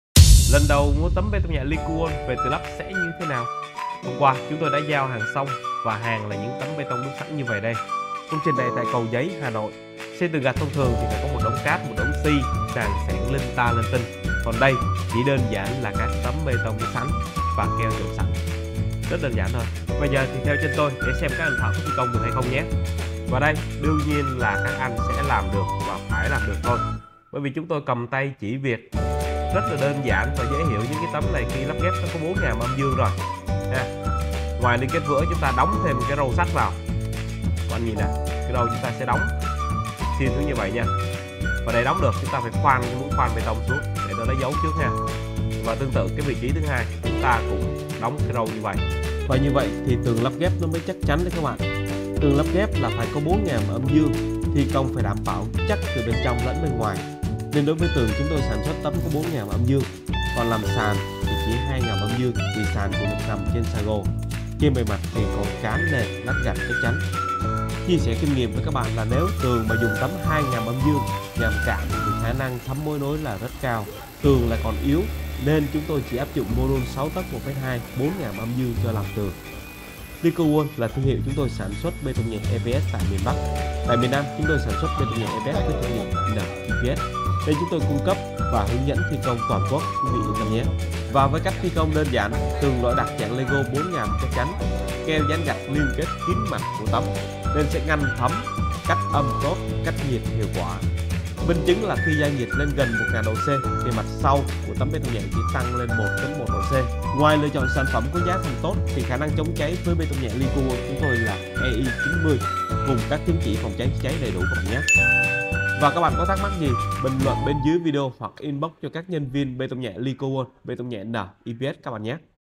Lần đầu mua bê tông nhẹ về tự lắp được không? Kích thước bê tông nhẹ eps miền bắc
Sx và cung cấp bê tông nhẹ miền bắc với thương hiệu Licowall
Sx và cung cấp bê tông nhẹ miền nam với thương hiệu N-EPS